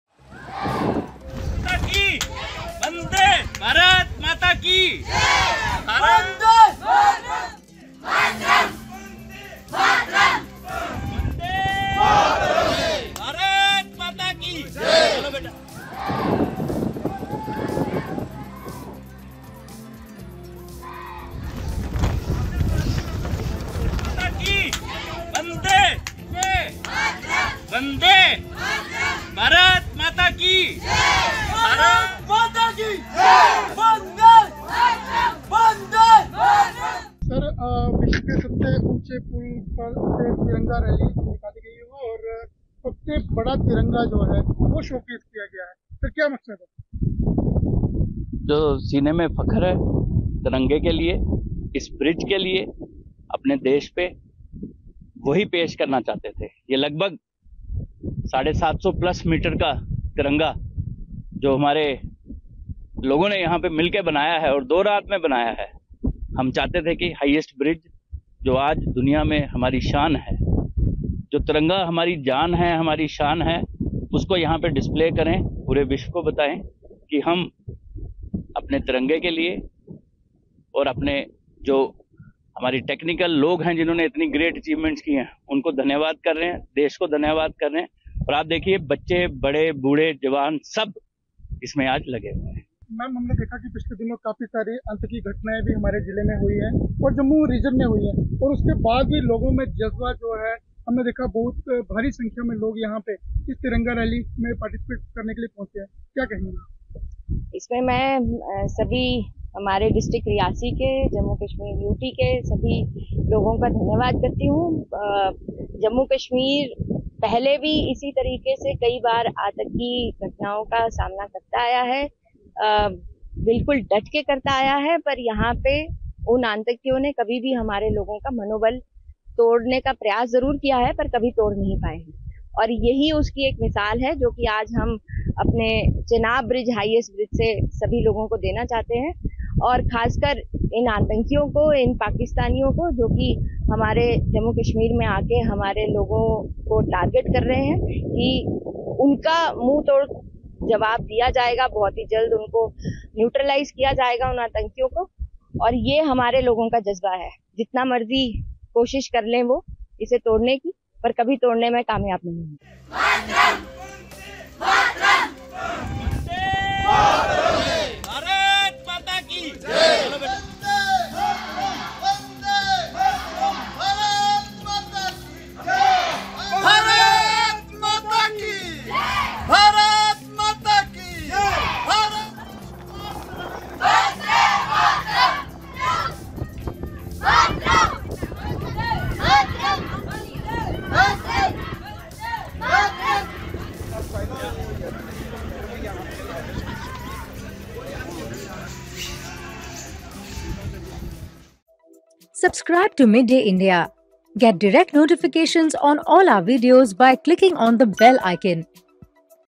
Independence Day 2024: Nationalism at its peak! Indian Flag hoisted on Chenab Railway bridge
As Independence Day approaches, the excitement in India is palpable. In Jammu and Kashmir's Reasi, people are especially enthusiastic. On August 13, a huge crowd gathered at the Chenab Railway Bridge, the highest railway bridge in the world. The atmosphere was filled with patriotic energy as people carried the Indian flag and chanted slogans like "Vande Mataram" and "Bharat Mata Ki Jai." This gathering showcased the deep sense of pride and unity among the people, reflecting their strong love for the country. The event at the iconic bridge, a symbol of national achievement and engineering marvel, added to the spirit of celebration and national pride.